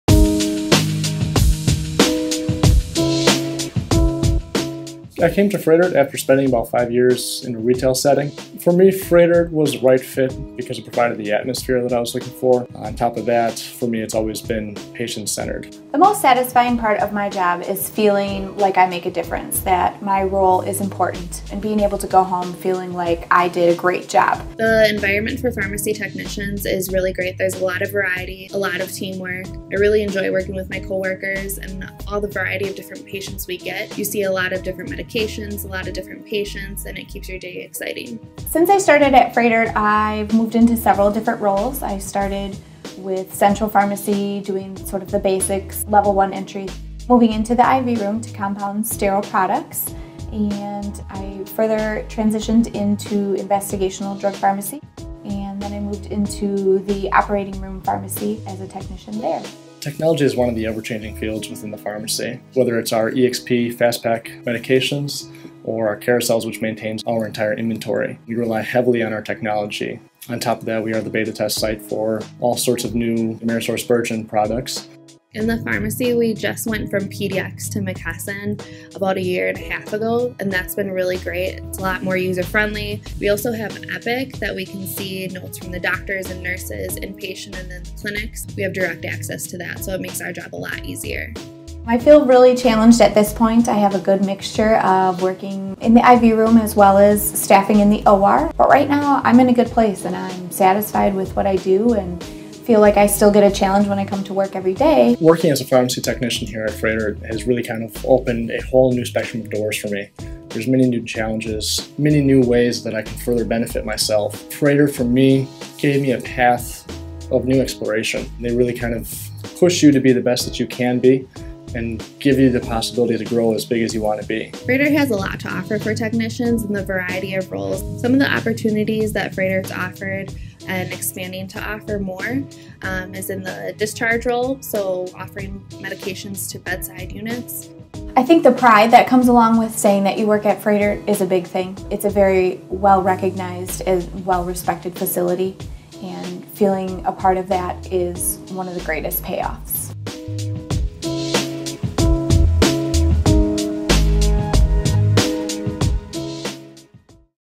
Pharmacy Tech Careers at Froedtert Health
Froedtert Health in Milwaukee, Wisconsin offers outstanding opportunities for pharmacy technicians. Here, you can work in an environment that fuels professional growth, collaboration and medical innovation. Find out what it's like to work here from pharmacy technicians, Megan, Mike and Valerie.